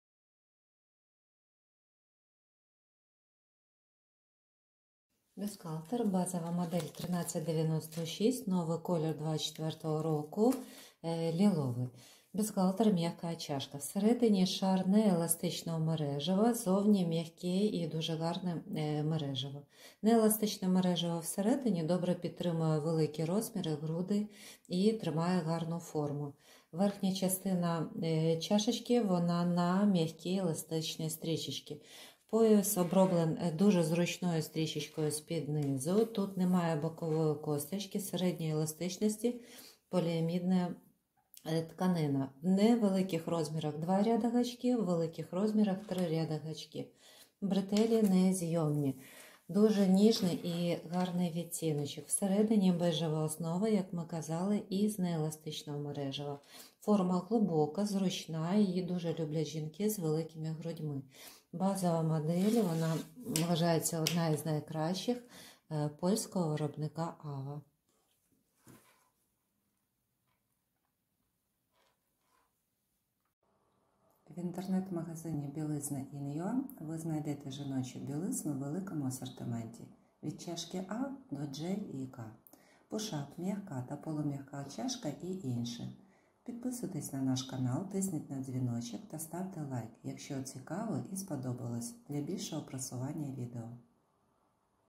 Ava Бюстгальте soft Cristal AV 1396 lily. BILIZNA.IN.UA
Avalingerie 1396 Crystal мереживний бюстгальтер на пишні груди.
Розкішний рожево-ліловий відтінок мережива.

Базова колекція польського бренду Ava.

Нижня частина чашки з двох шарів мережива, добре підтримує великі груди.

Верхня частина з м'якого еластичного мережива для більш щільного розташування на жіночих грудях.

Бретели незнімні. Форма глибока та комфортна.

Ava вишукана жіноча білизна у великому асортименті для великих розмірів.